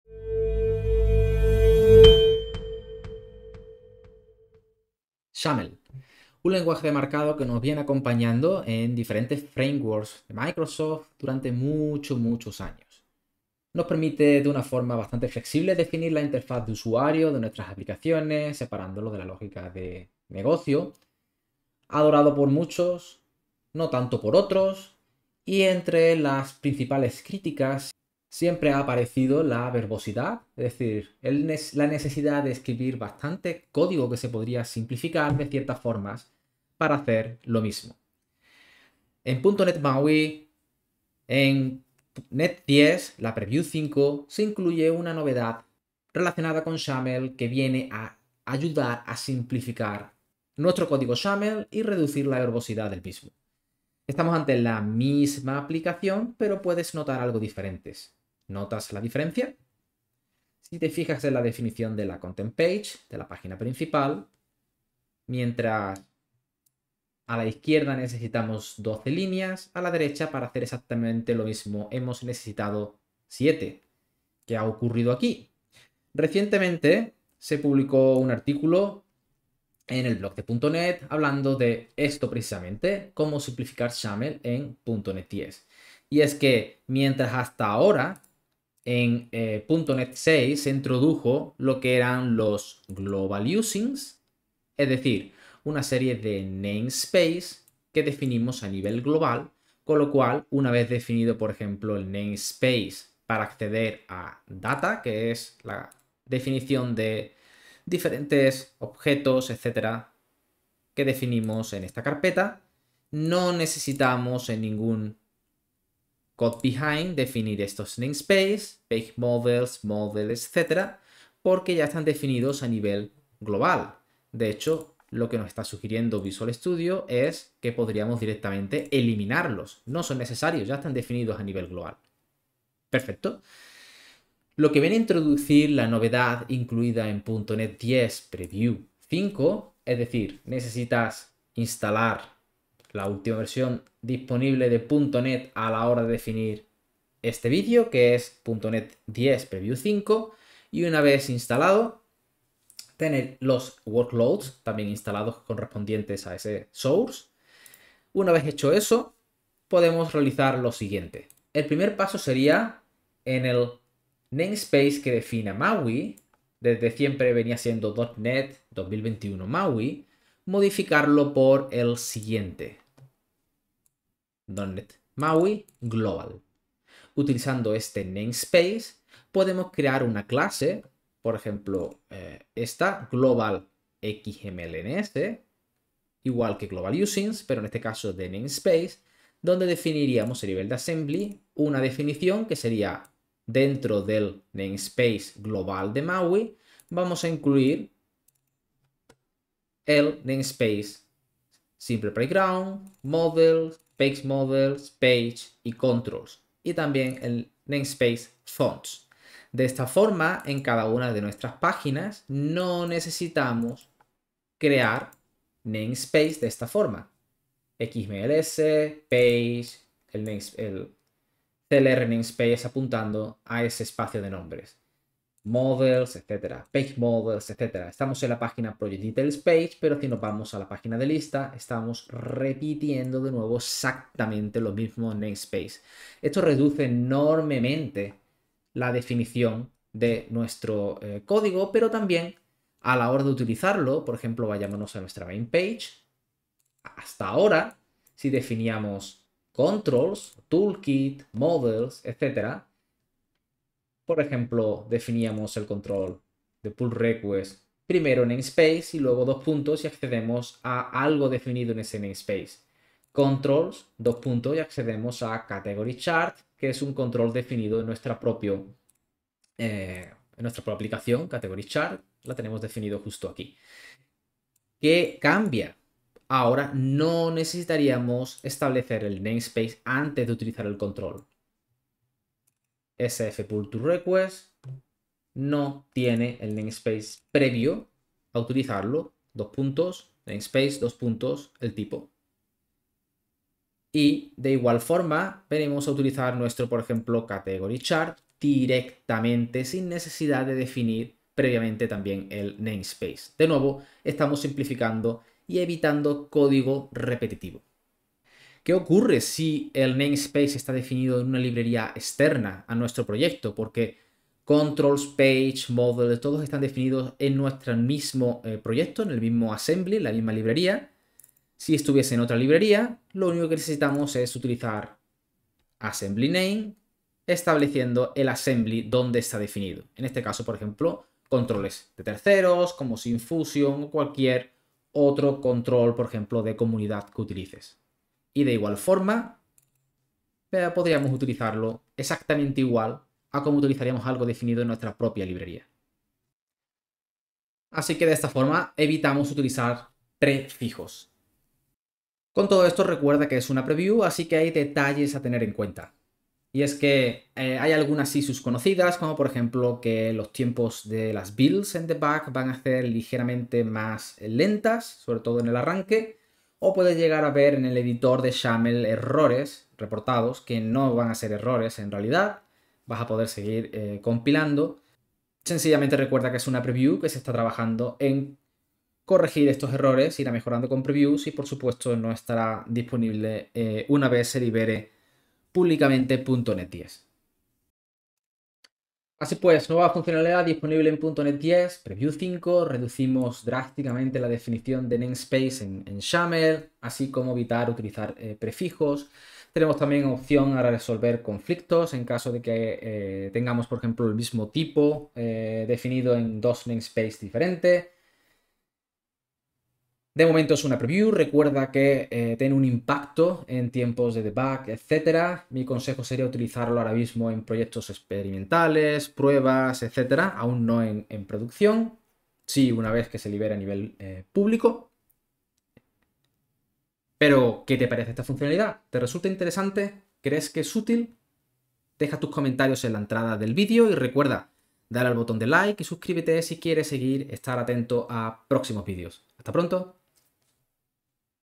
Simpler XAML in .NET MAUI 10
.NET 6 introduced global and implicit usings for C# which greatly reduced the using statements at the head of many C# files. Now in .NET 10 starting with Preview 5 we are introducing the same for XAML so you can declare your namespaces and prefixes in a single file and use them throughout. In fact, you can now omit the use of prefixes altogether!

Become a channel member to enjoy benefits and access more content.